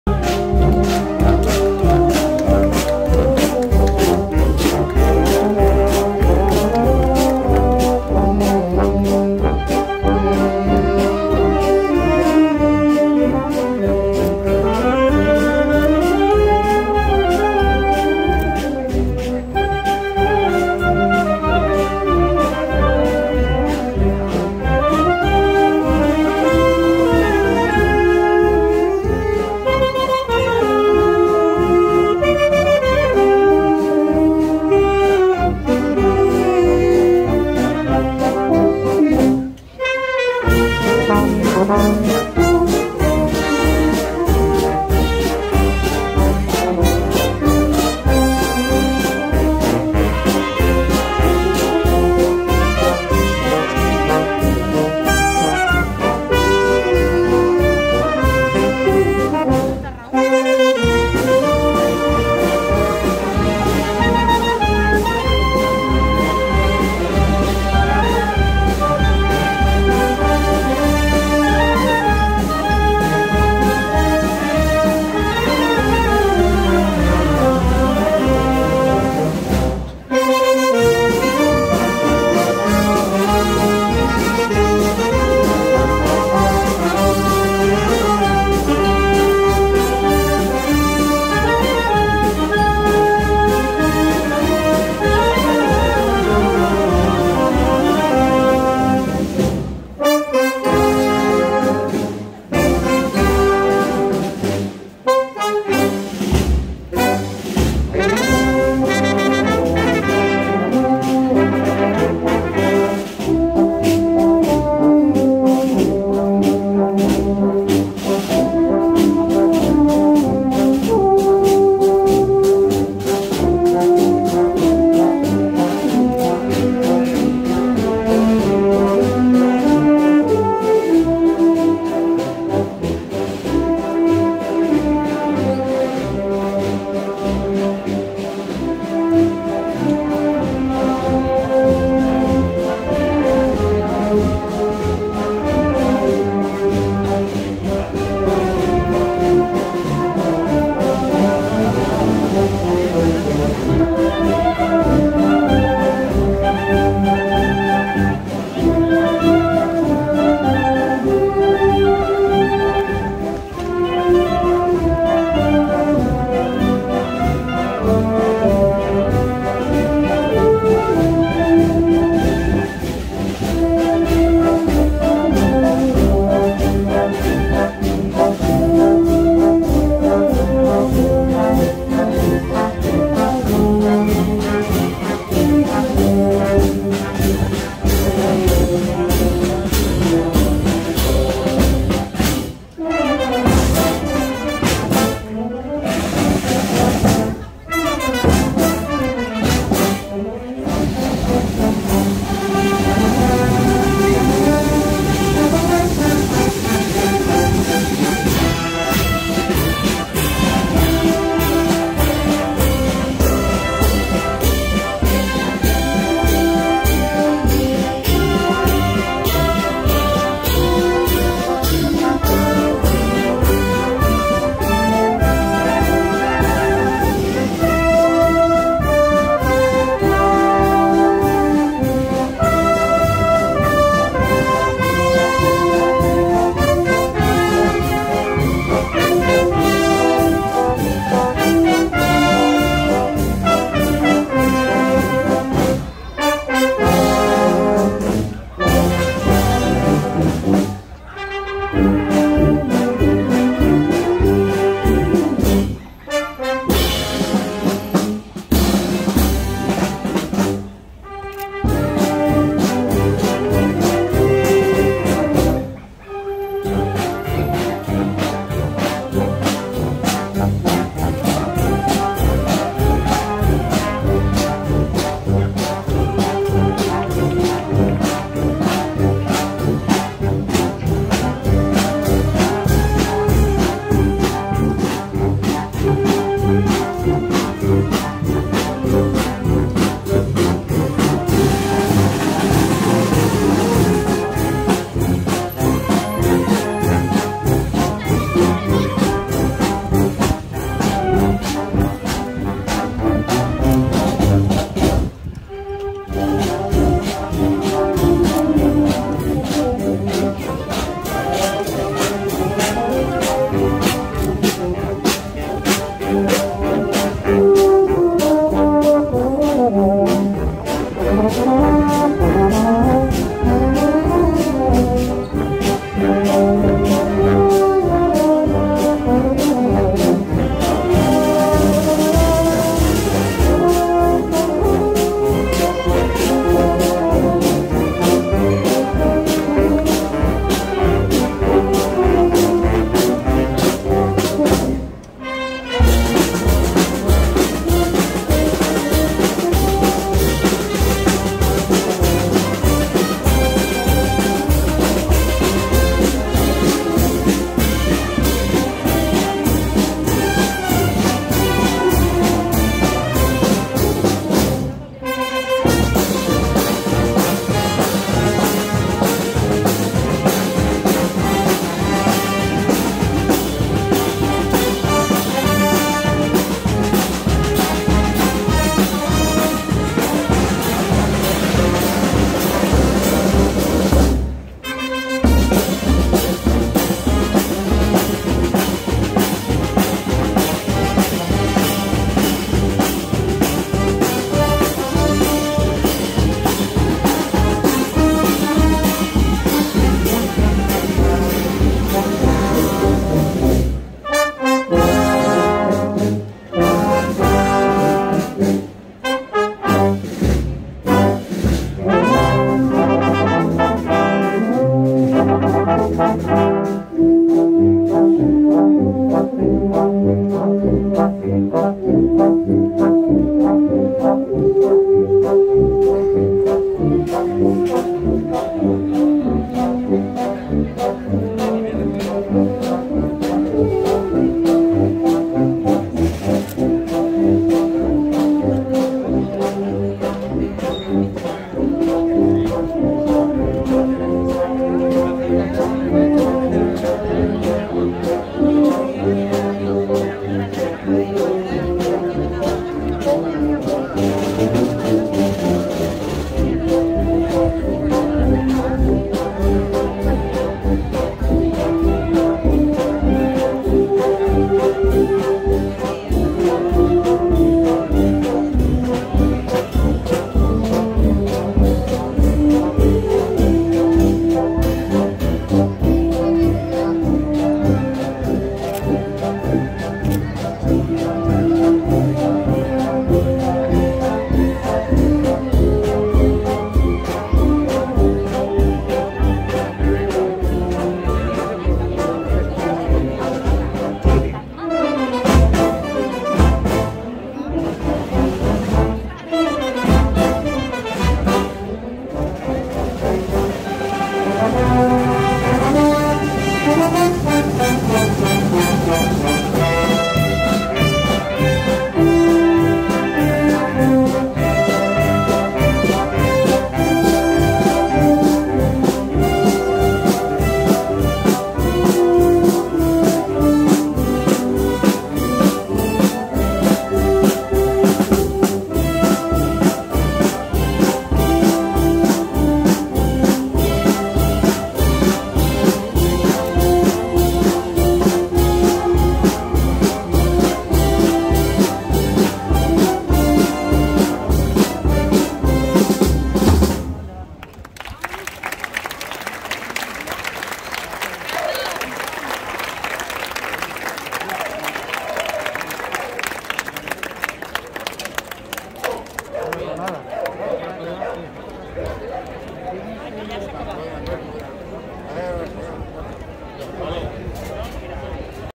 Pasacalles de la Banda “La Flor de la Mancha”
Pasacalles previo al concierto de la  Banda “La Flor de la Mancha” de La Puebla de Almoradiel (Toledo) del como cierre XIX Festival Internacional de Bandas de Música “Ciudad de Torrevieja”.